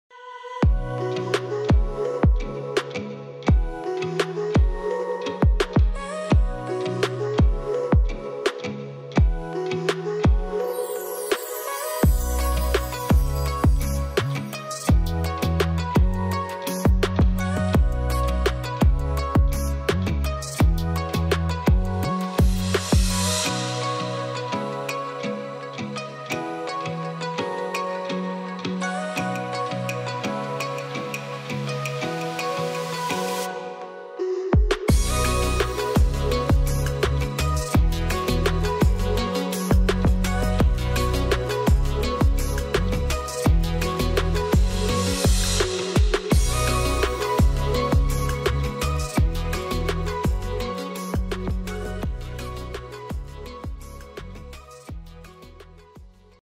TO LET - Southernwood, EL - RXCF-6107 - Video Tour
RE/MAX Border presents this recently renovated unit in Quarry View. There are two spacious bedrooms with built-in cupboards, 1 full bathroom, and a neat open plan kitchen and lounge. There is a well-kept yard off the lounge with high walls for privacy and security.

Rental is exclusive of water and electricity.